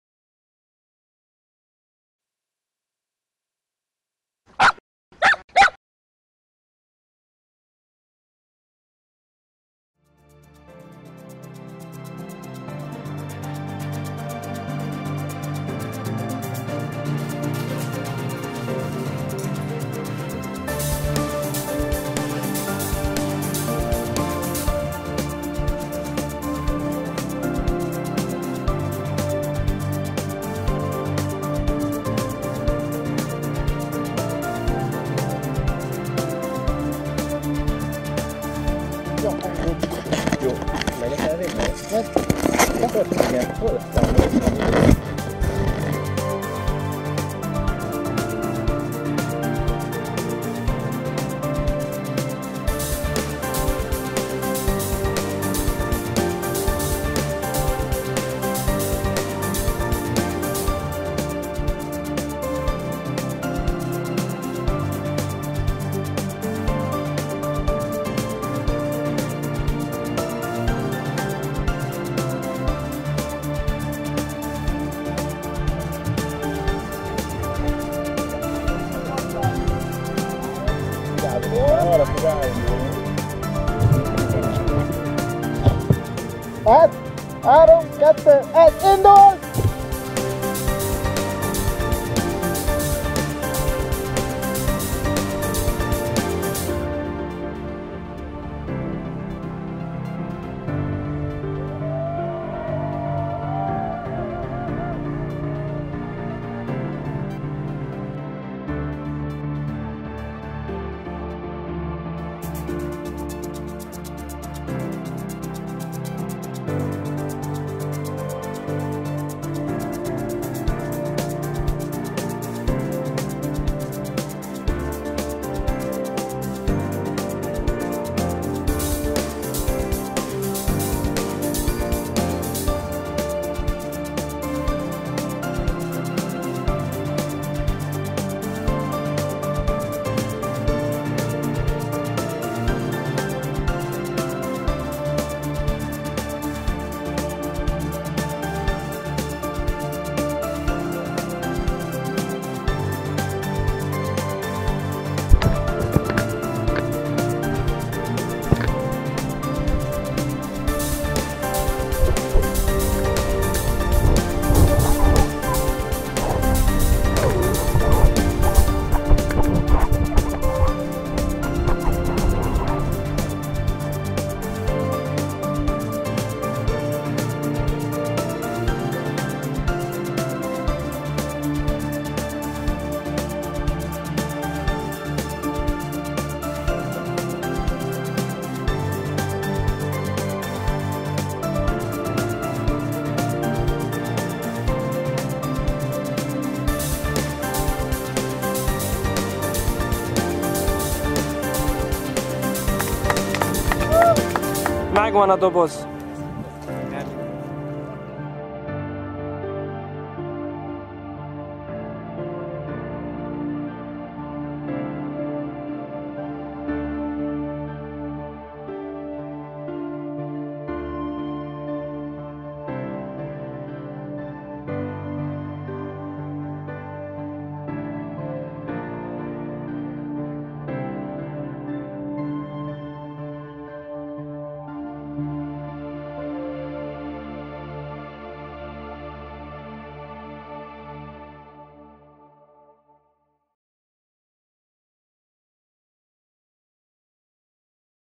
Puli goes high altitude
Gagarin űrrepülésének 54. évfordulóján a Puli is magasba merészkedett. Mintegy 36 km-ig vitte fel hasznos terhét, a Nescafé dobozt.

On the 54th anniversary of Gagarin's flight, Puli flew up to 36 km. The Nescafé can payload contained a small coffee shrug piece as well. Nescafé pointed out that the worldwide production of coffee is not able to cover the increasing consumption demand, and so coffee might become a luxury item soon.